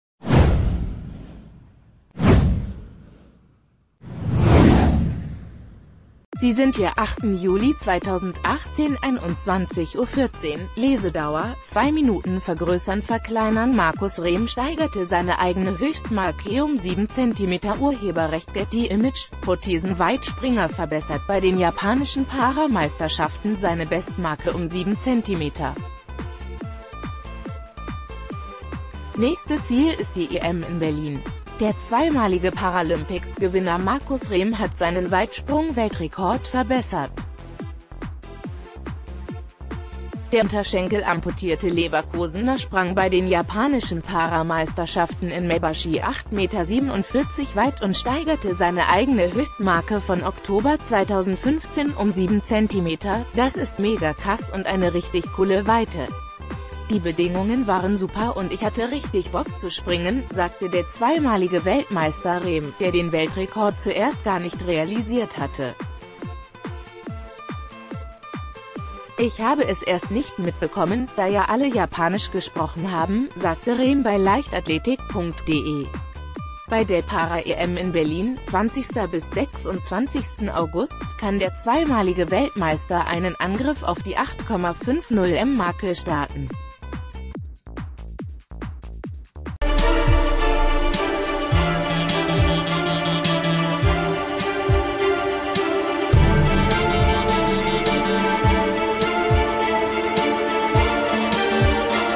Prothesen-Weitspringer Marcus Rehm verbessert Weltrekord
Sie sind hier: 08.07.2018 / 21:14 Uhr / Lesedauer: 2 Minuten vergrößernverkleinern Markus Rehm steigerte seine eigene Höchstmarkeum sieben Zentimeter © Getty Images Prothesen-Weitspringer verbessert bei den japanischen Para-Meisterschaften seine Bestmarke um sieben Zentimeter. Nächste Ziel ist die EM in Berlin. Der zweimalige Paralympicsgewinner Markus Rehm hat seinen Weitsprung-Weltrekord verbessert. Der unterschenkelamputierte Leverkusener sprang bei den japanischen Para-Meisterschaften in Mae...